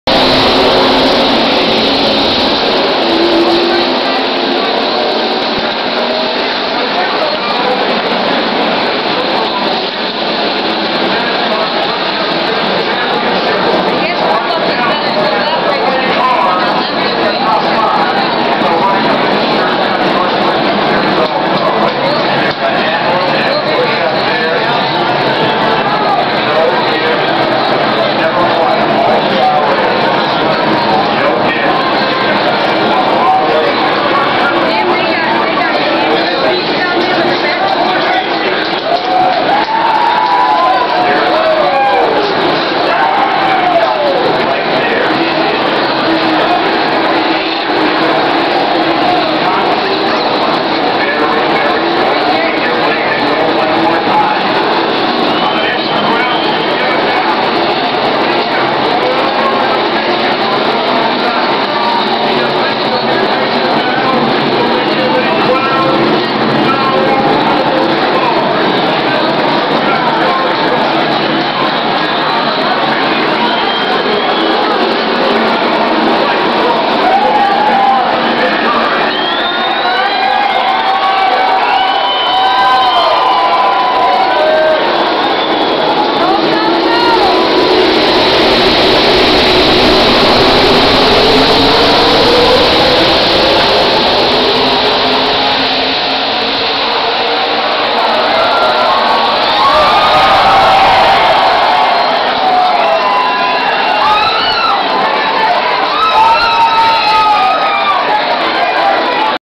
2010 NASCAR Sprint All Star Race Part 18
Start of final 10 lap segment